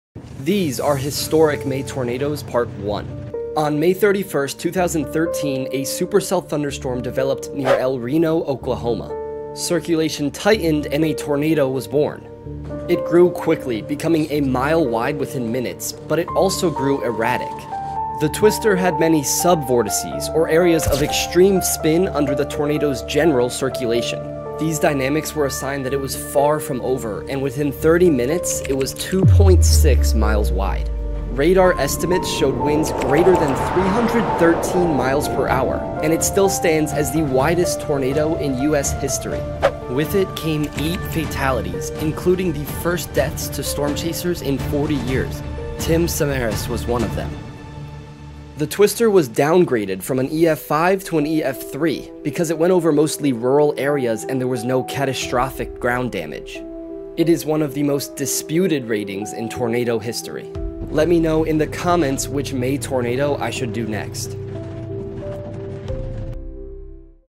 HISTORIC MAY TORNADOES - Part 1🌪️
May is NOTORIOUS for having some of the worst tornadoes worldwide, especially across the United States. In this series, I'll break down the science and stories behind historic May tornadoes based on which ones YOU request. Comment and let me know!🌪️